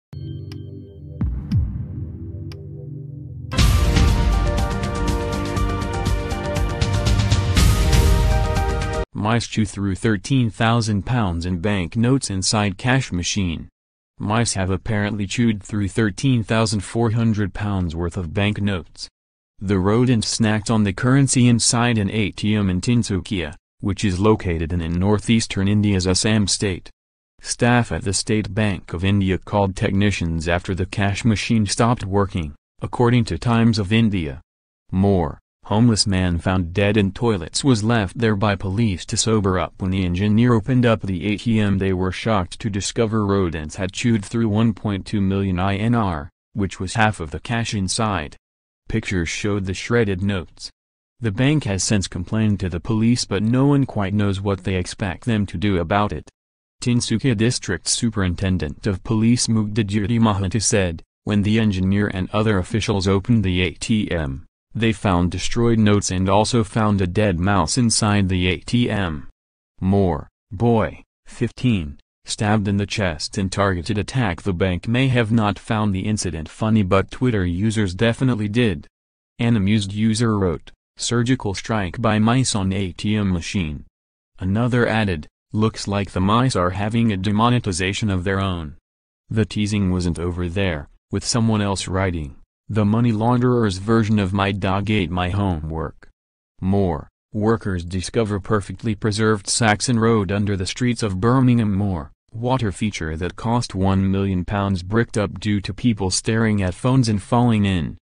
Mice chew through £13,000 in bank notes inside cash machine
Mice chew through £13,000 in bank notes inside cash machine. Mice have apparently chewed through £13,400 worth of bank notes.

The rodents snacked on the currency inside an ATM in Tinsukia, which is located in in north-eastern India’s Assam state.

Staff at the State Bank of India called technicians after the cash machine stopped working, according to Times of India.

MORE: Homeless man found dead in toilets was left there by police ‘to sober up’

When the engineer opened up the